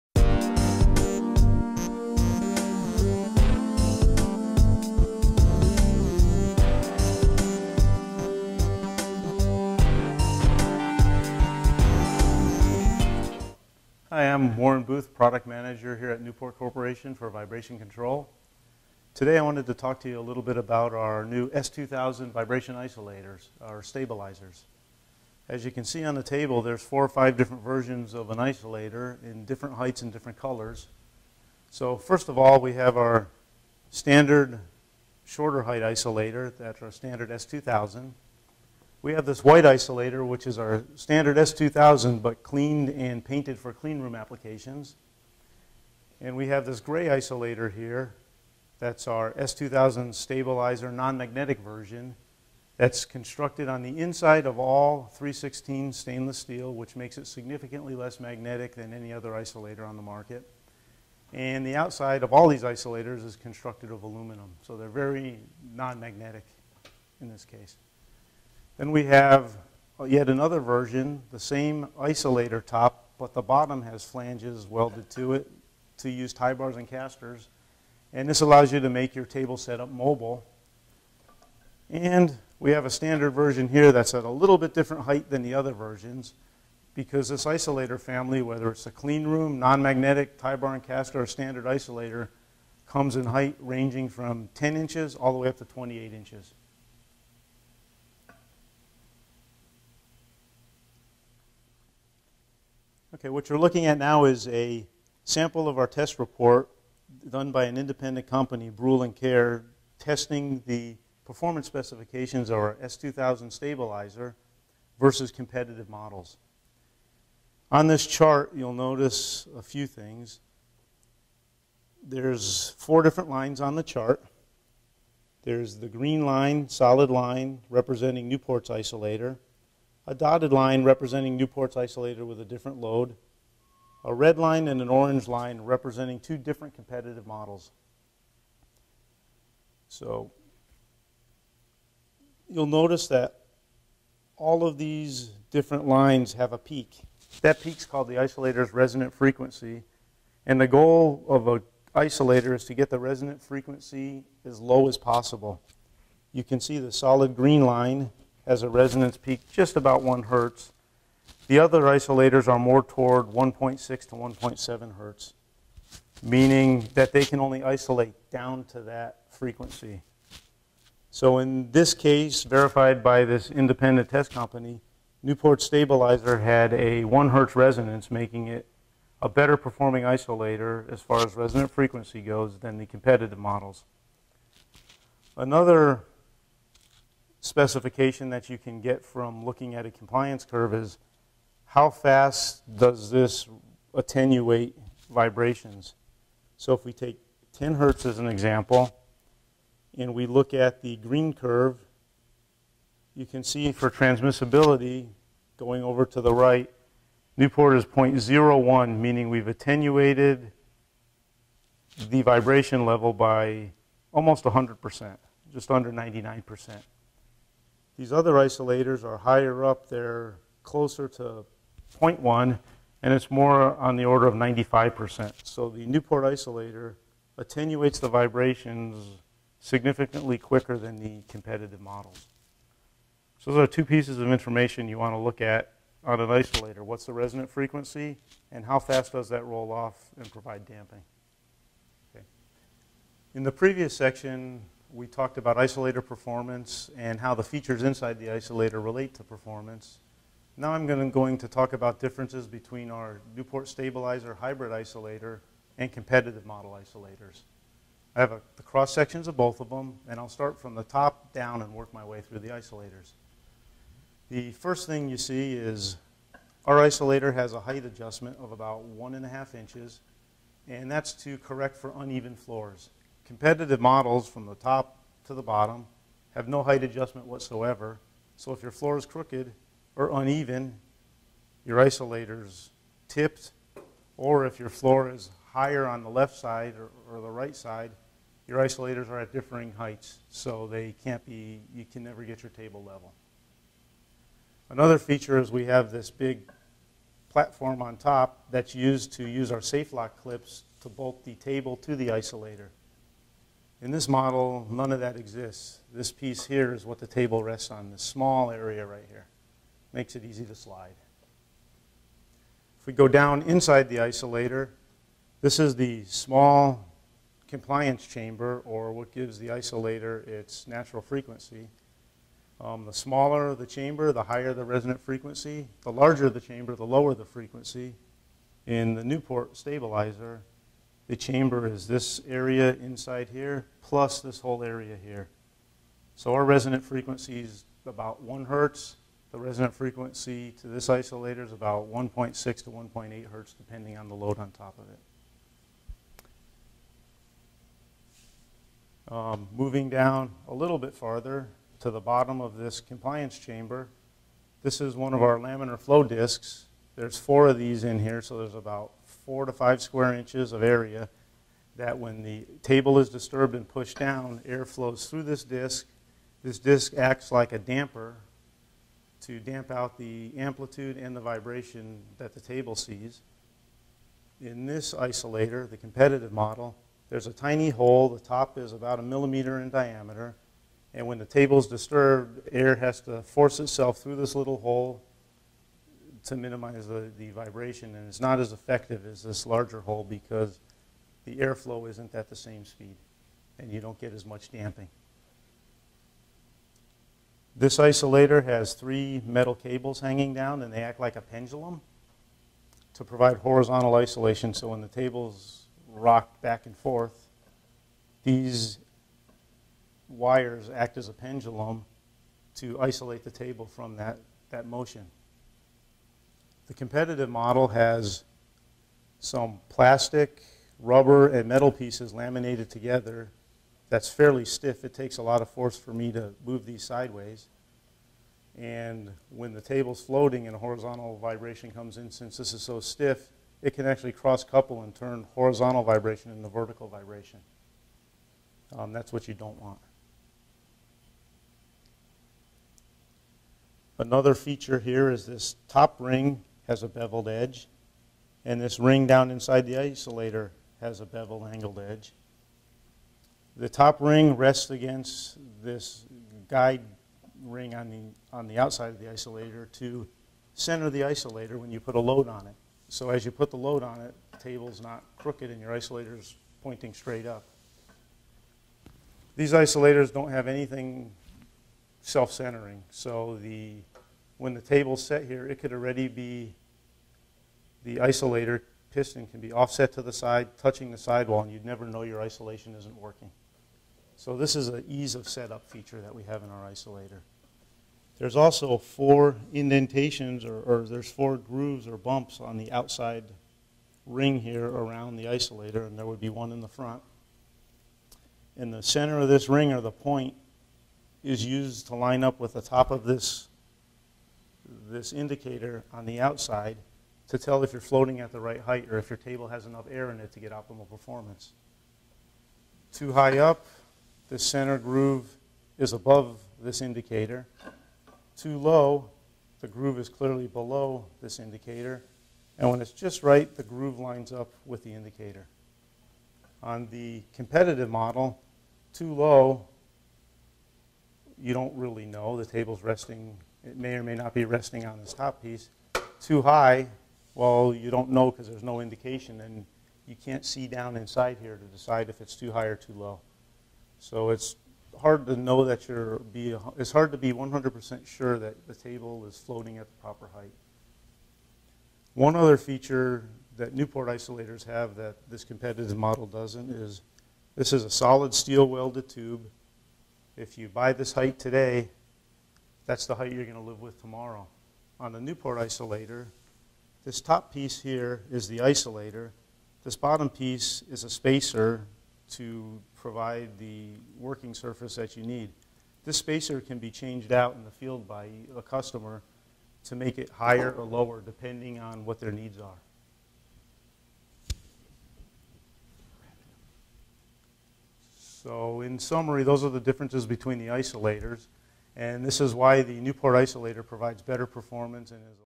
S-2000 Stabilizer™ Pneumatic Isolators with Automatic Re-Leveling Overview
S-2000 Stabilizer™ Pneumatic Isolators with Automatic Re-Leveling Overview with detailed discussion of technical specifications and construction